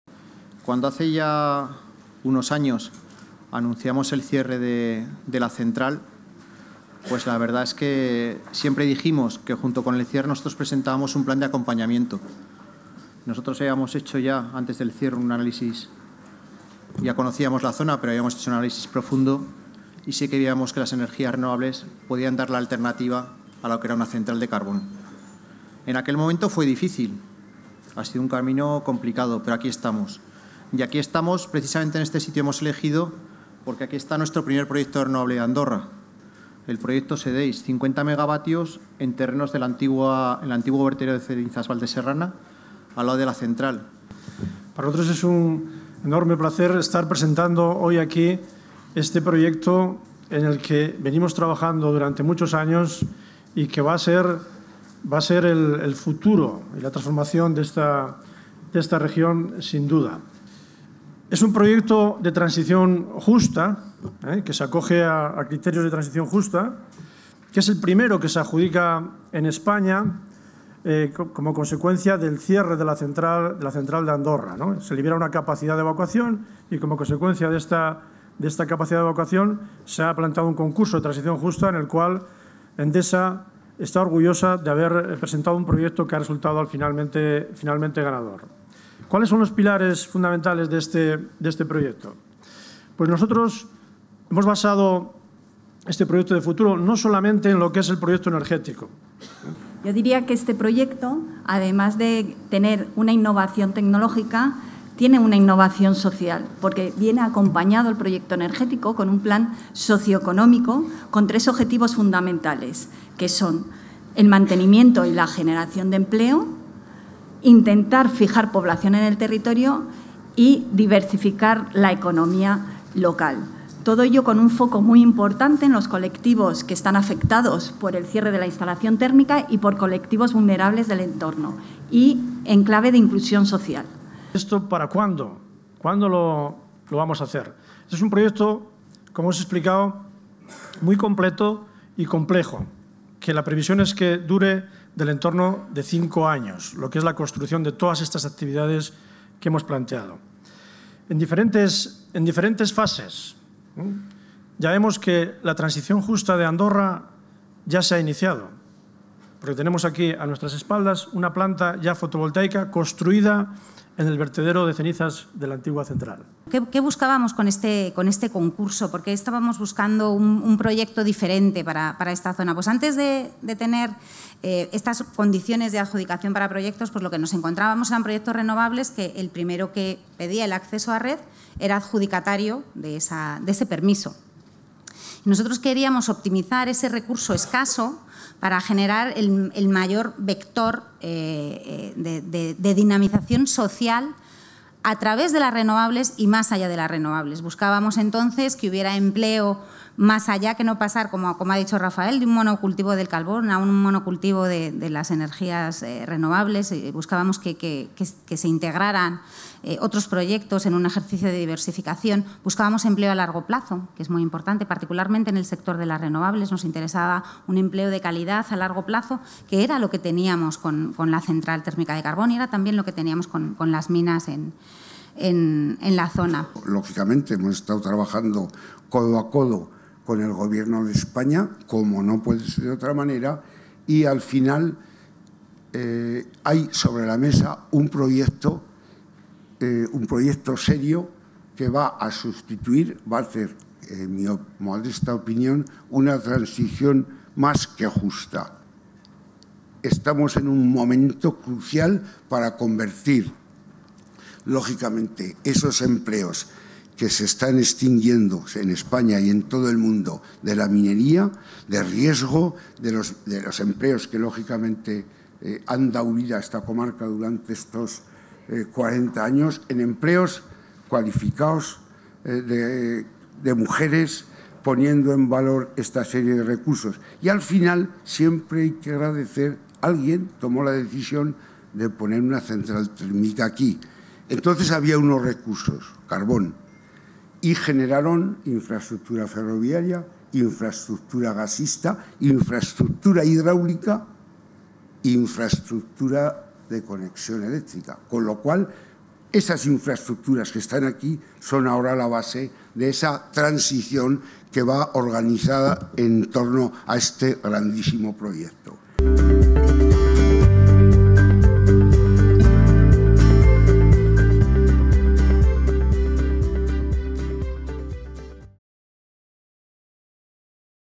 ENDESA presenta su PLAN de FUTURO para ANDORRA
El director general de Generación de Endesa, Rafael González, y la directora general de Sostenibilidad de la compañía, María Malaxechevarría, han presentado en la mañana del 15 de diciembre de 2022 en Andorra el proyecto renovable y su plan de acompañamiento socioeconómico para el entorno de la Central Térmica de Teruel. Un futuro a cinco años que dejará 500 empleos fijos y duraderos en la zona.